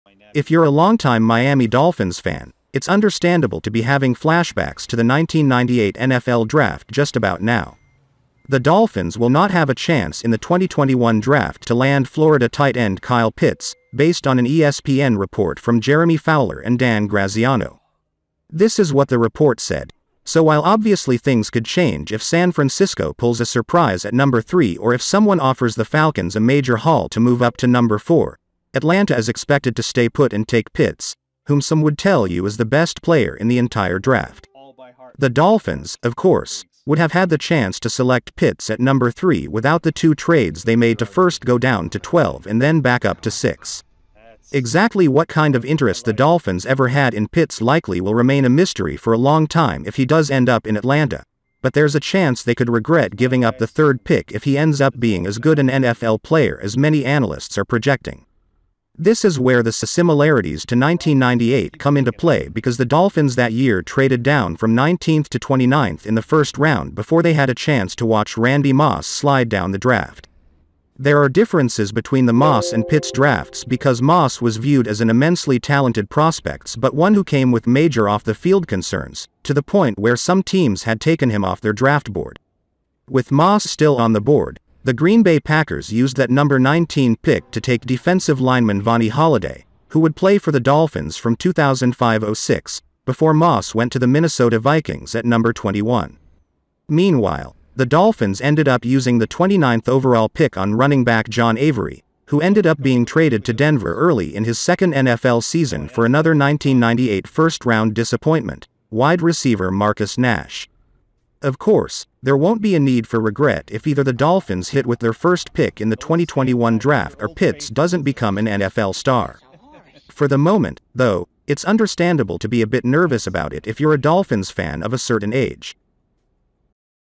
Miami Dolphins news Today: “Judgement Day” , Kyle Pitts is not belong to the Dolphins!
Miami Dolphins news Today: “Judgement Day: NFL 2021 draft” , Kyle Pitts is not belong to the Dolphins!